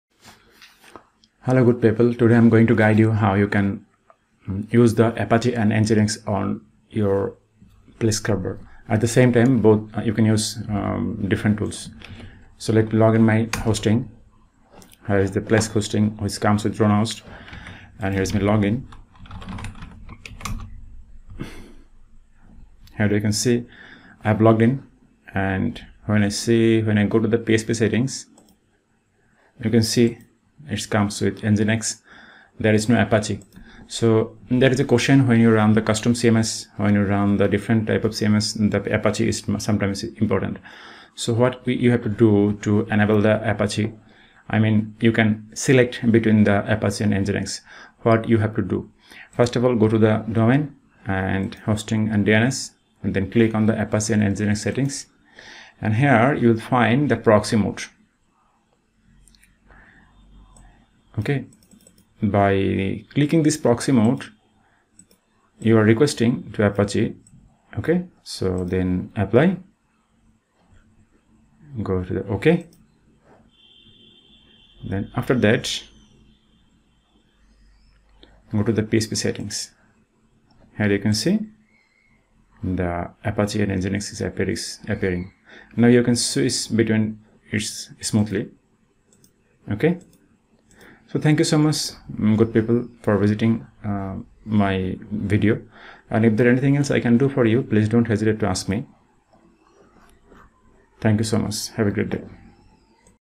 How to switch between Nginx to Apache: Plesk | 2022
In this video, I describe how you can switch between Nginx to Apache (FPM application served by Nginx to FPM application served by Apache). This video will save you time in web search.